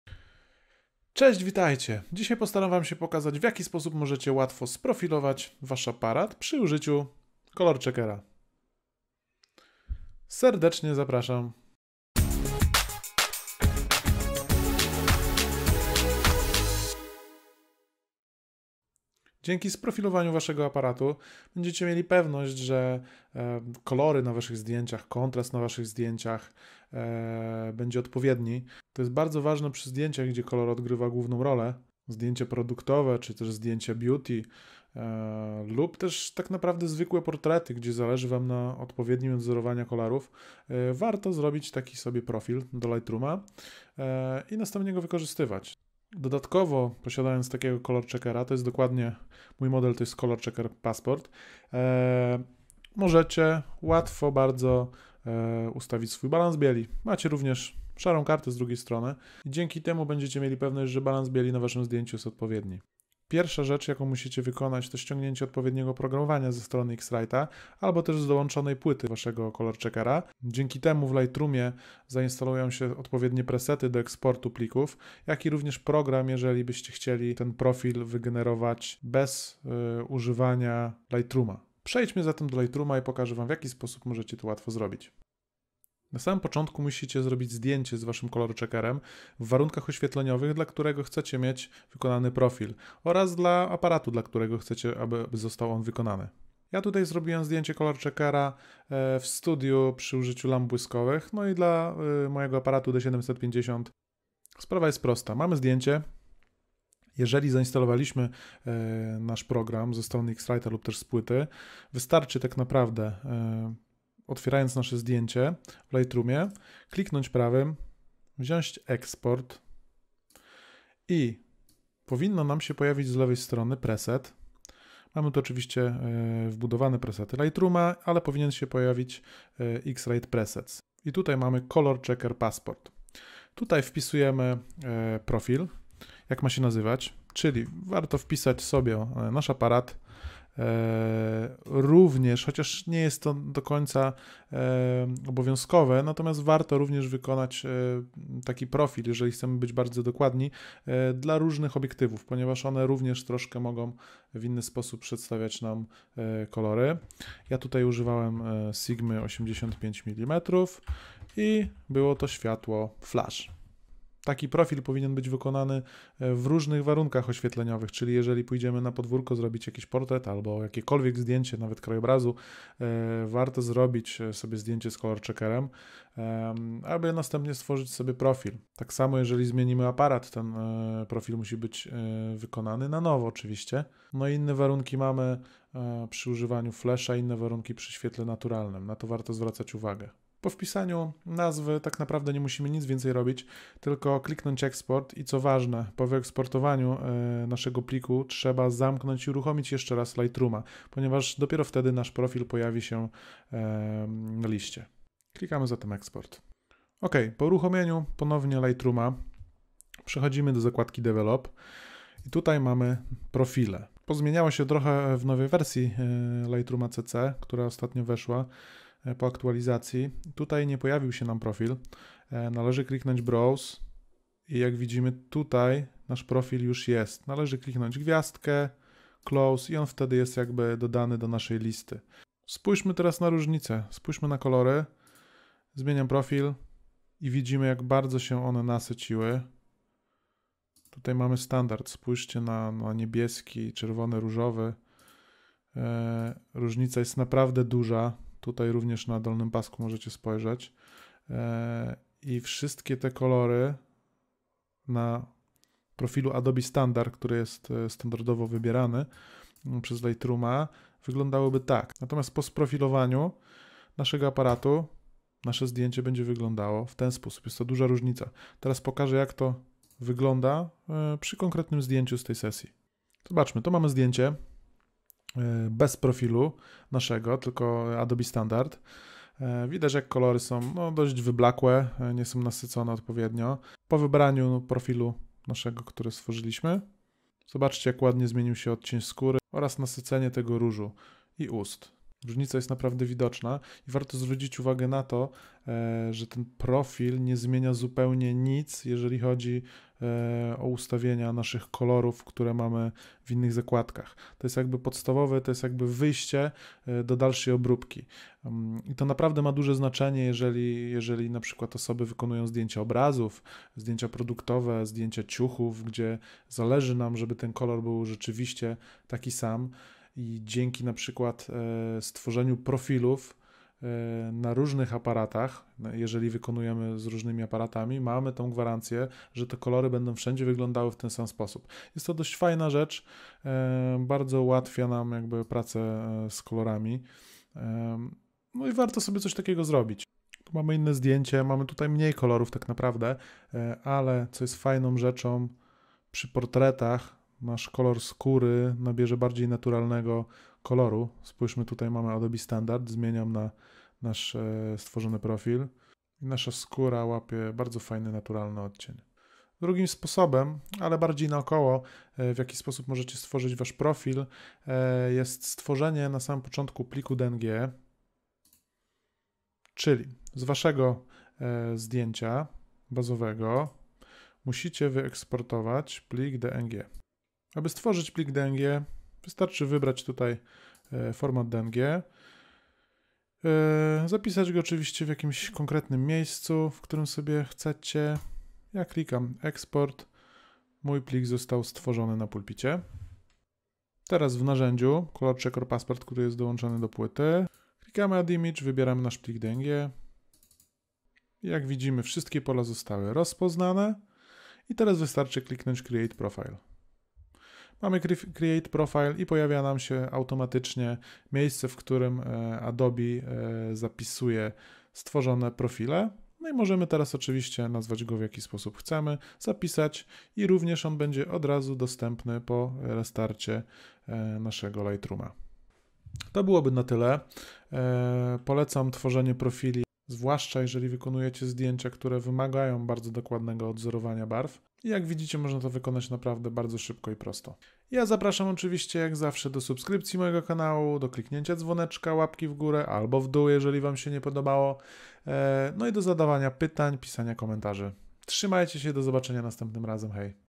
15. Profilowanie aparatu przy pomocy Color Checkera
W tym odcinku pokazuję jak łatwo możecie sprofilować Wasz aparat przy pomocy Color Checkera w Adobe Lightroom.

Zapraszam wszystkich serdecznie do subskrypcji oraz zadawania pytań.
Pozdrowienia

❗❗❗ Zapraszam na moją grupę na FB: Świat Fotografii Live ❗❗❗